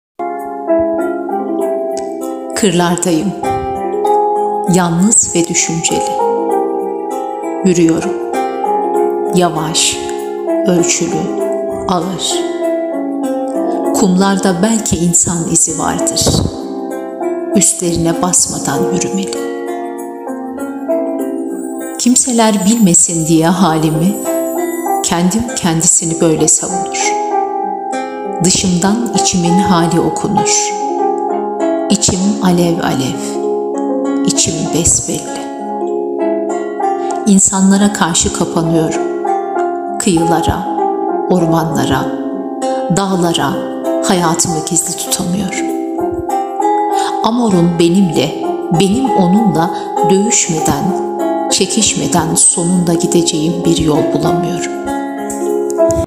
Hazin Yalnızlık Francesco Petrarca
Kırlardayım, Yalnız ve düşünceli;
Yürüyorum, yavaş, ölçülü, ve ağır;
Kumlarda belki insan izi vardır
üstlerine basmadan yürümeli.